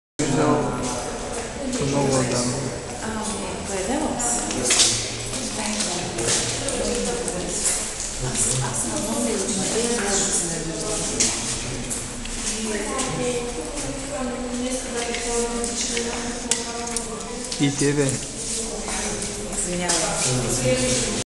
XVII-GARDDIQUE BIENALLE, VARNA-CITY - 01.08.2013.DSCN0773
OPENED FROM THE MUST YOUTH MAJOR OF THE CITY OF VARNA HIS HIGHNESS IVAN PORTHNICH. AND ALSO SOME REMARKS FROM DIMITAR MANOLLOFF MEMORIAL EXPOSSITION.